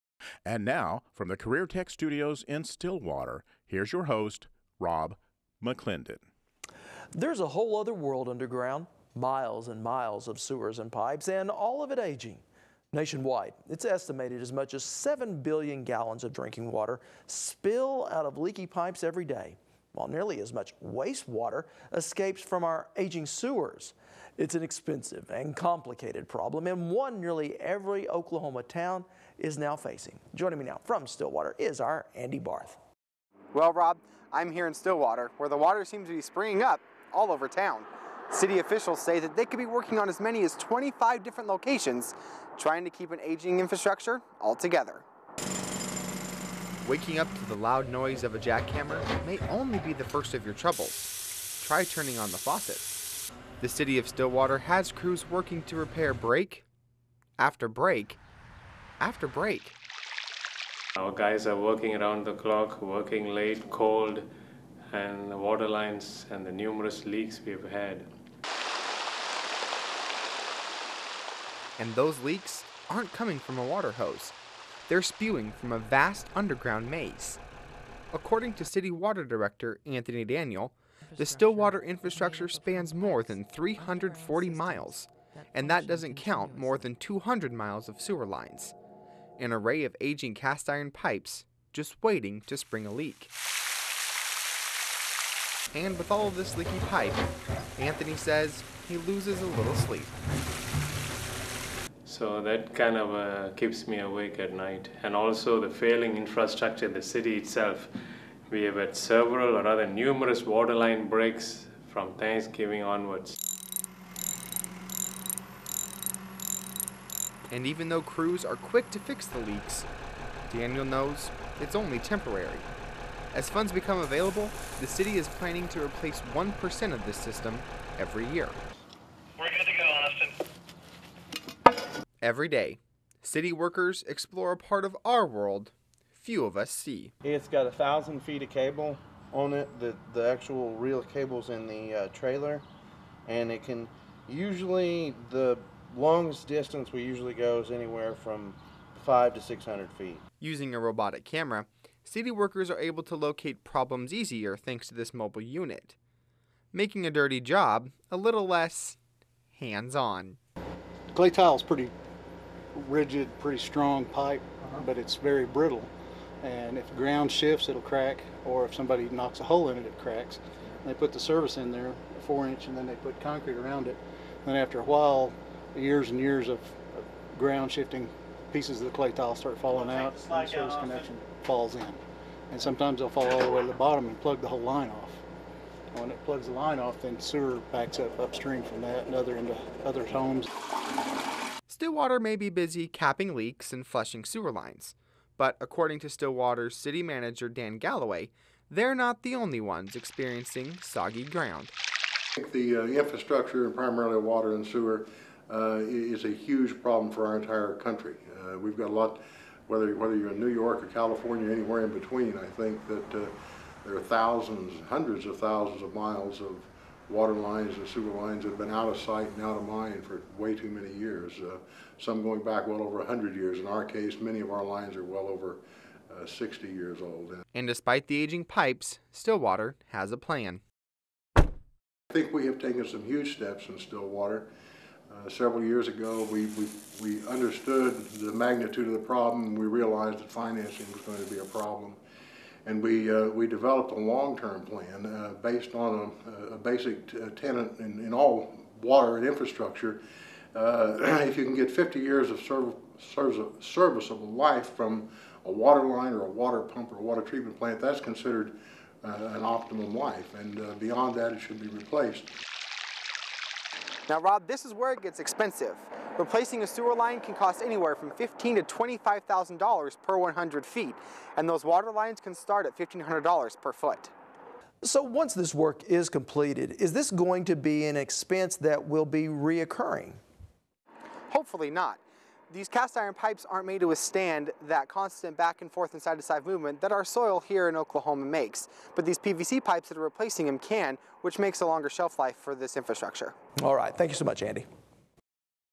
Underground Infrastructure Around the World
There is a whole other world underground; miles and miles of sewers and pipes, and all of it aging.